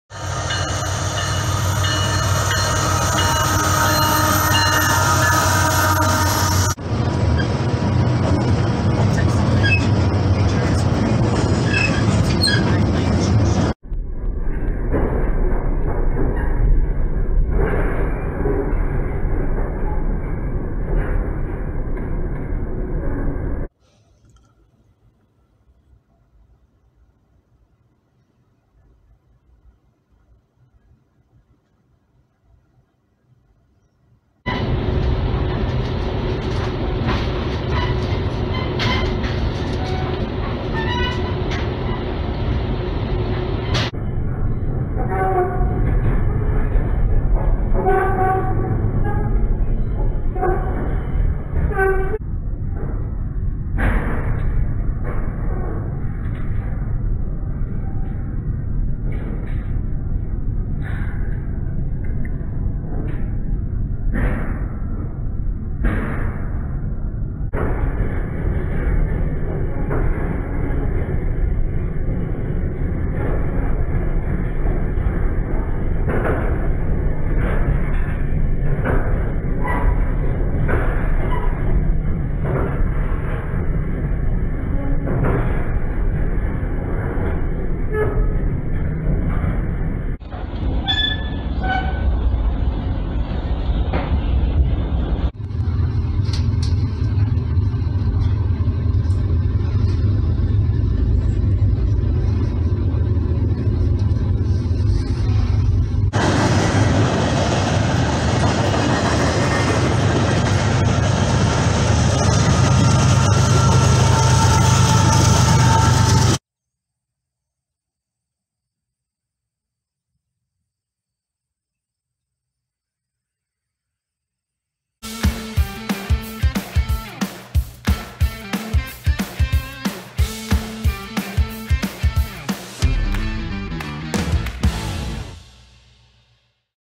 CN Engines around Chicago
Hey, fellow railfans!  We've got a compilation of various CN locomotives from around the Chicago area; there's even a DPU and a freight loco being used by Amtrak!  Enjoy the view!